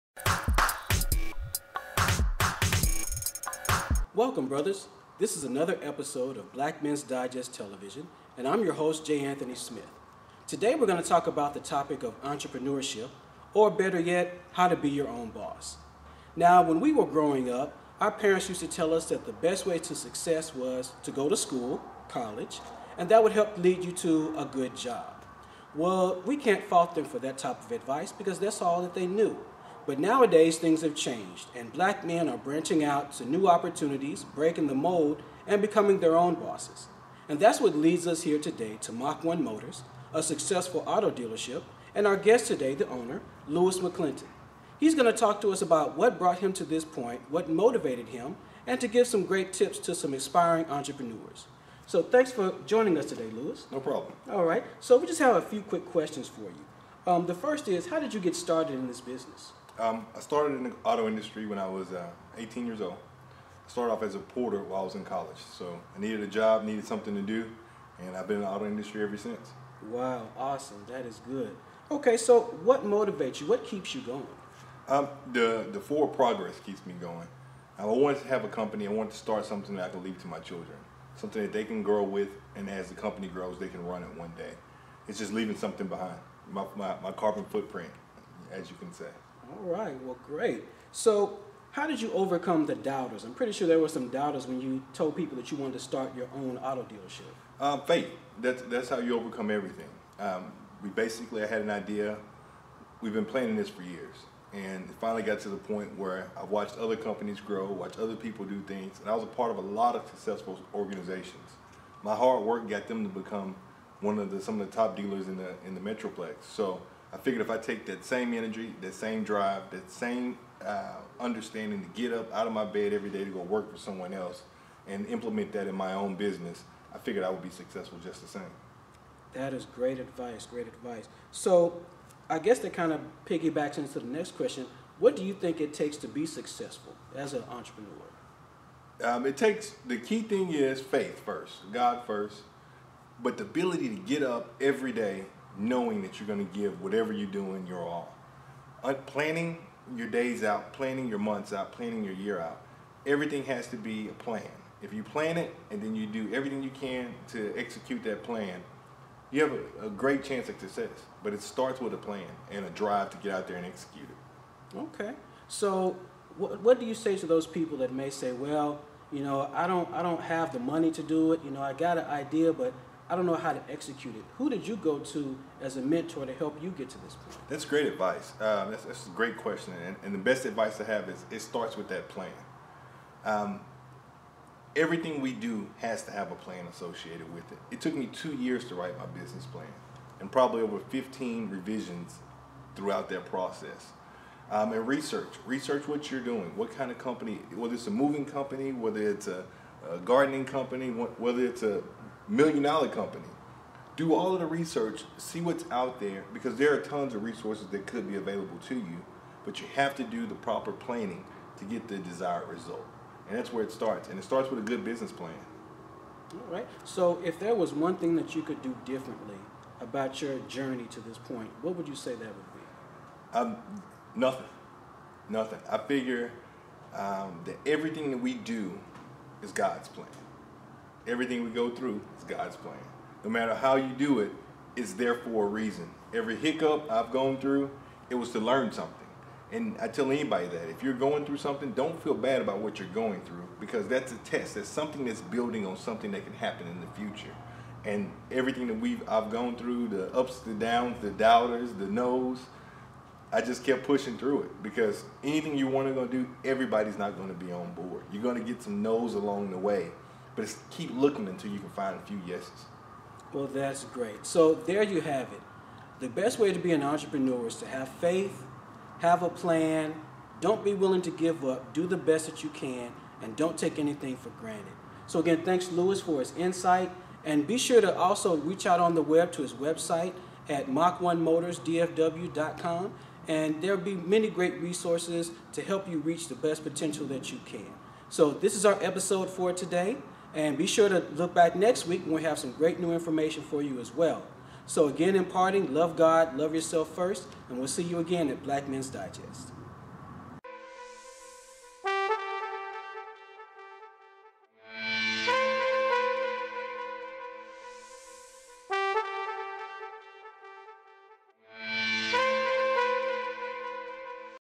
How to be a successful Entrepreneur BMD S1E2
Black Men's Digest discusses the topic of Entrepreneurship with Louis McClinton of Mach 1 Motors Black business owner gives advice to help those who want to be their own boss.

black men's digest